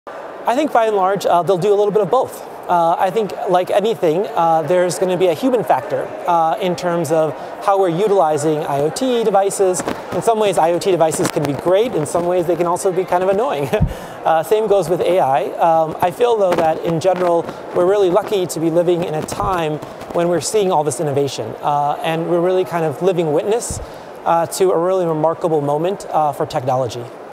Global IGF 2016: Manu Bhardwaj on Pros and Cons of AI and IoT
Manu Bhardwaj, Senior Political Advisor of Technology and Internet Policy, Office of the Under Secretary, U.S. Department of State, shares his answer to the question, "How will the Internet of Things and AI improve life; how are they also disrupting things?" Recorded at the Global Internet Governance Forum 2016 in Guadalajara, Mexico, by the Imagining the Internet Center, an initiative of Elon University, North Carolina, USA.